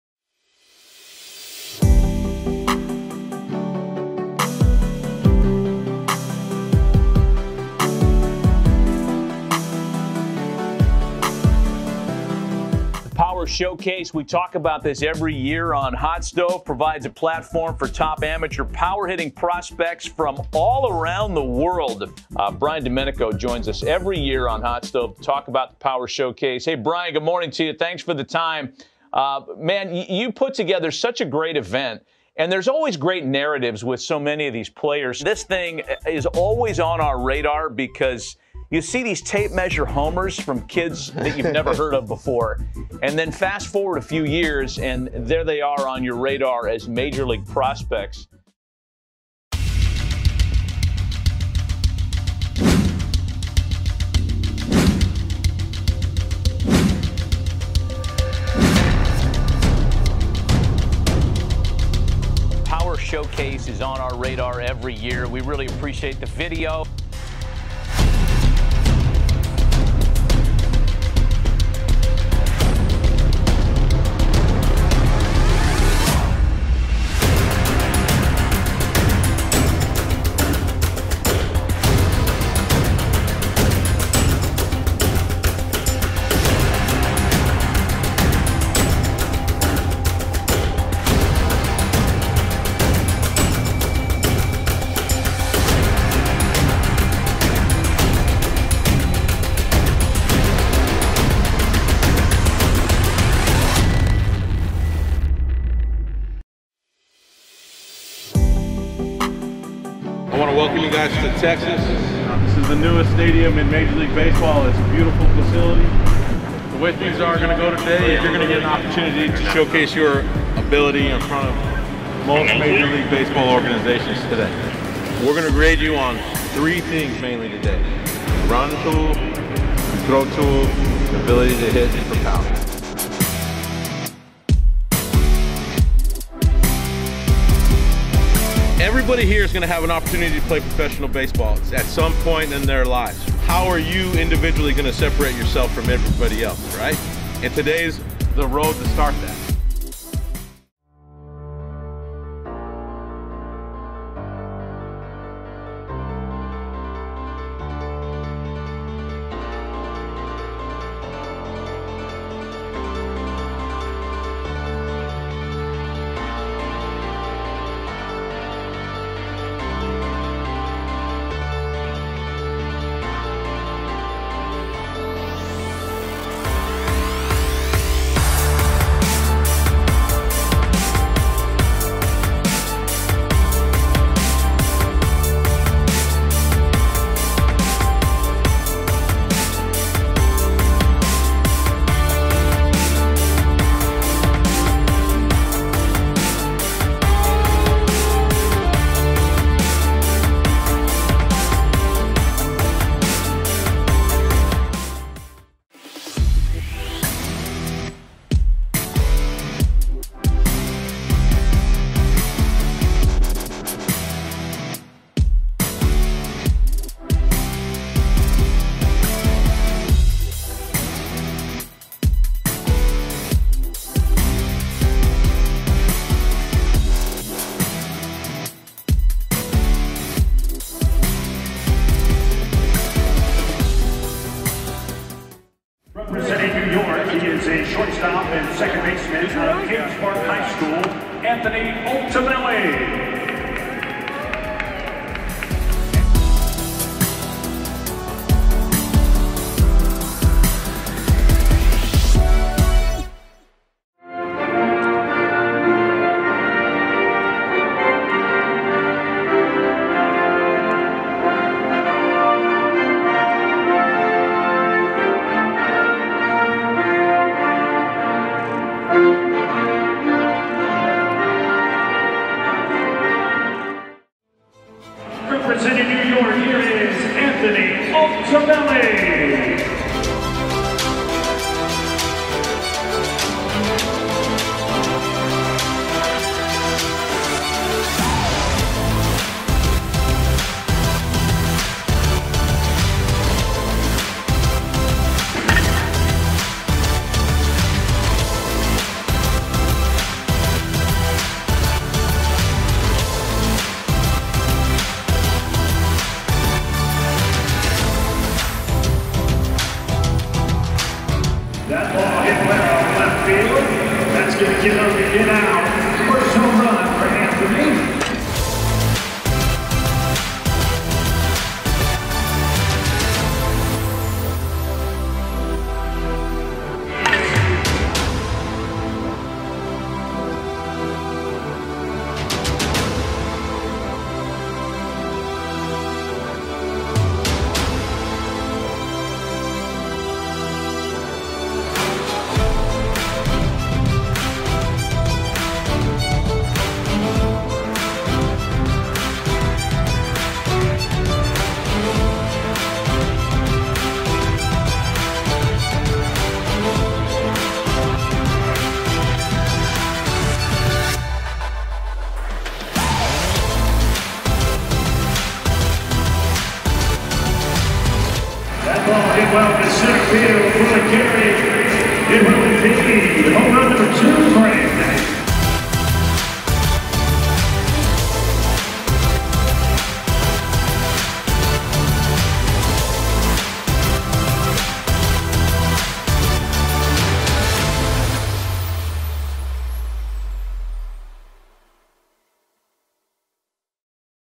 Top New York Prospect - 14U Anthony Altobelli - 5th National POWER SHOWCASE - Globe Life Field 2020.
RESULTS:
Offensive Game MVP
Home Runs - 2
Longest Distance - 409ft
HitTrax Top Exit Velocity - 97.2MPH